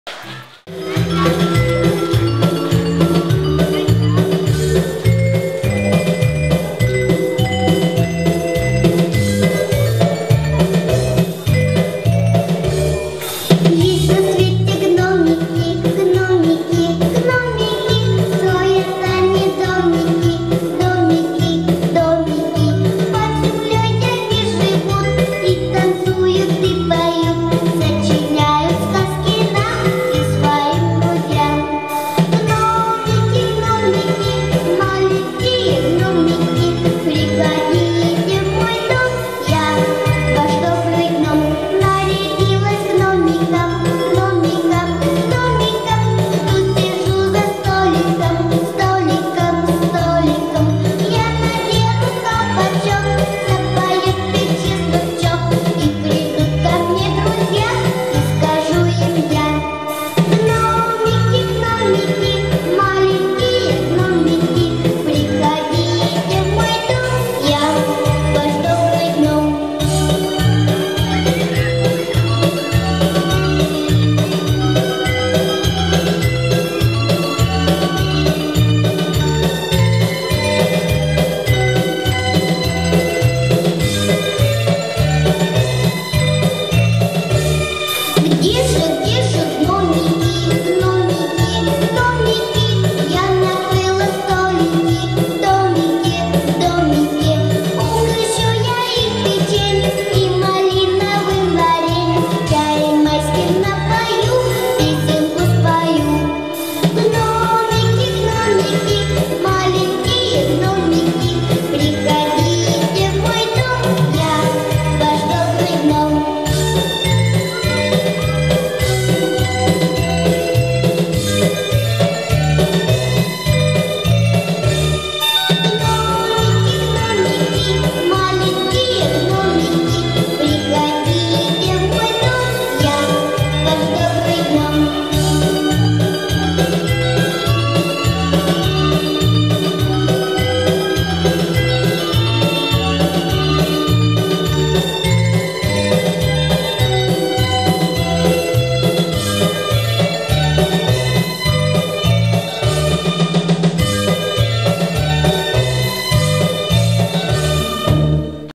BANANA 2013 MAY CLIP02
Клуб Банана. Хвалимся своими учениками! Клип 2. Маленькие гномики